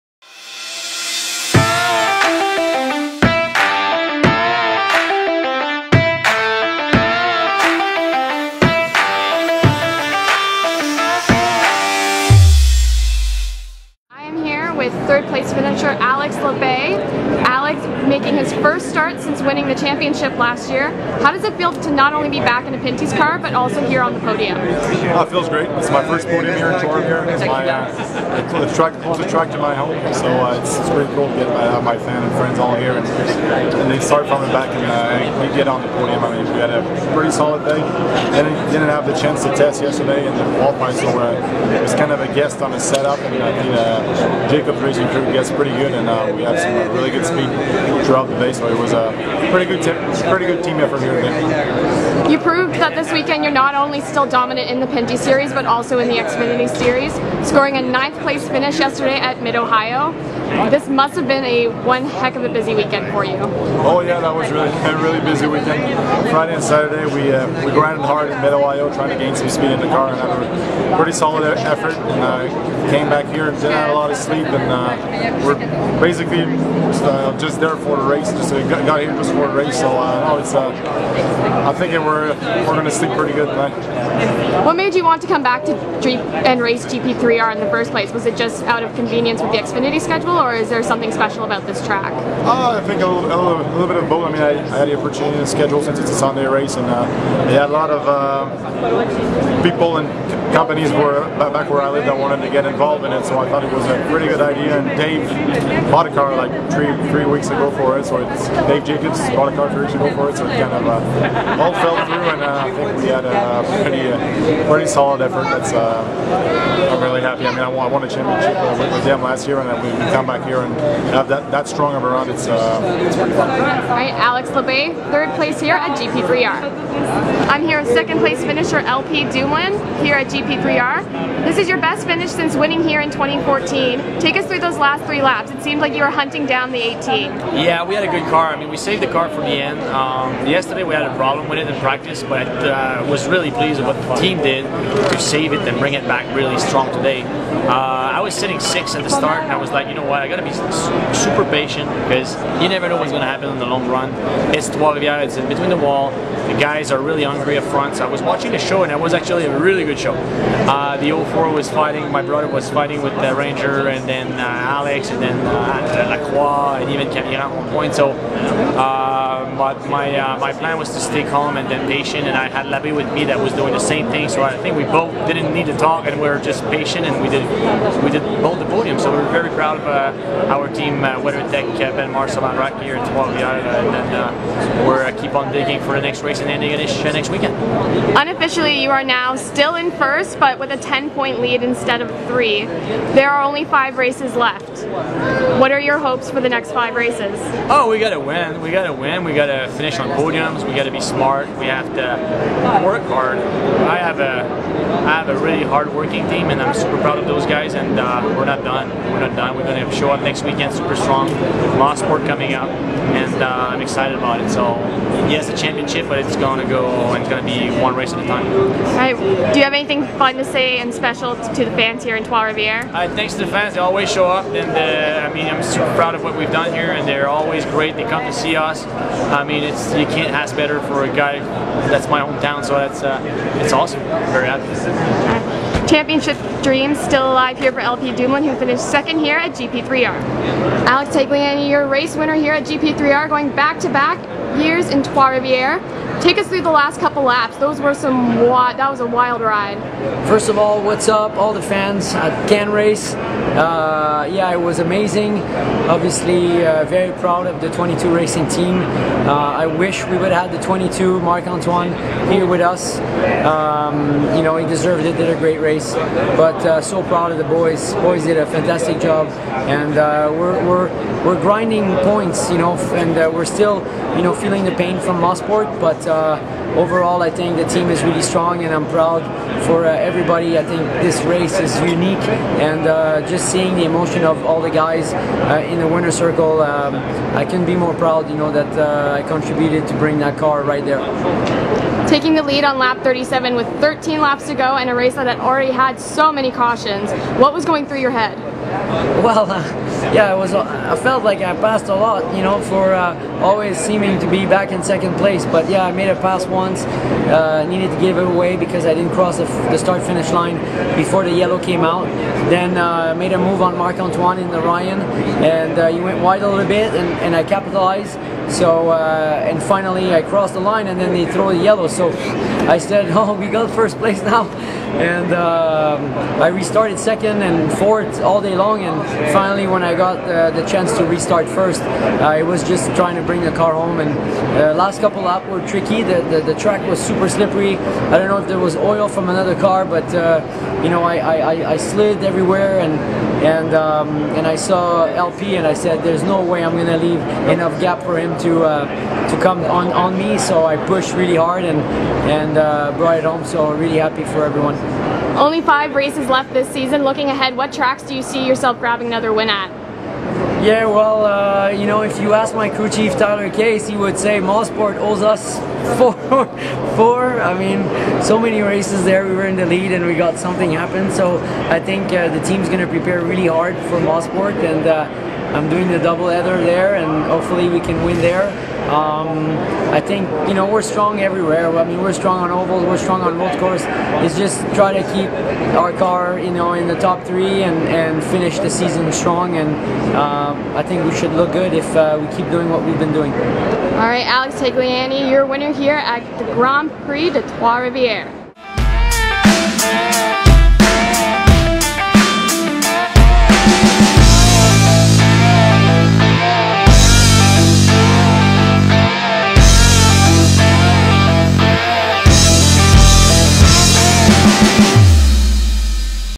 CANrace Post-Race for GP3R 2018
Your podium for Le 50 Tours Can-Am at GP3R: Alex Tagliani, L.P. Dumoulin and Alex Labbe. See what they had to say after today’s race.